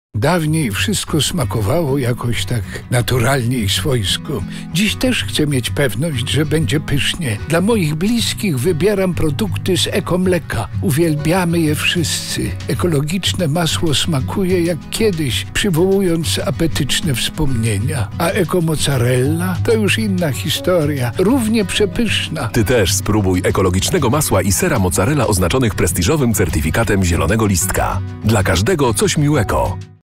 Spot reklamowy Polmlek 30s - Dla każdego coś MiłEKO!
Spot realizowany w ramach kampanii informacyjno-promocyjnej realizowanej ze środków UE